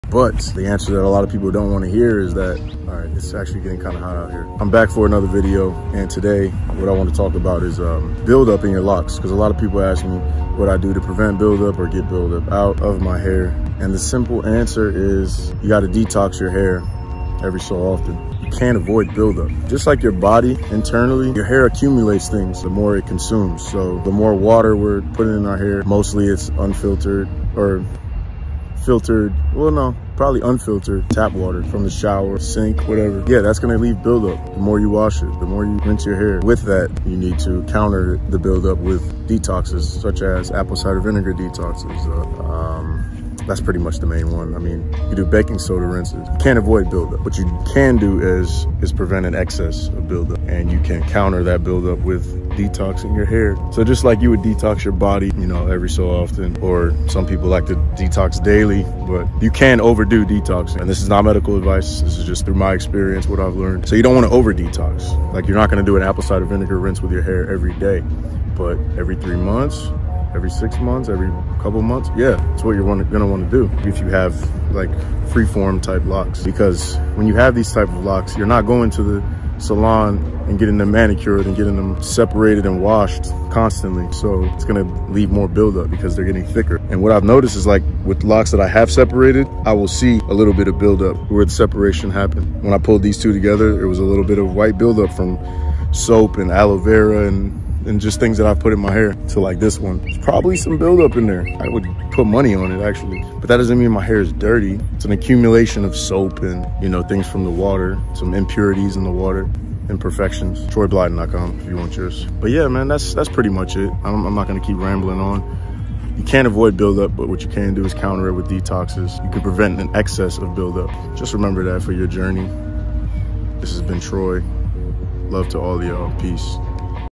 How To Prevent Buildup in Freeform Locs | 📍Adelaide, Australia
Can you actually prevent buildup in your freeform locs? These are my thoughts on the topic..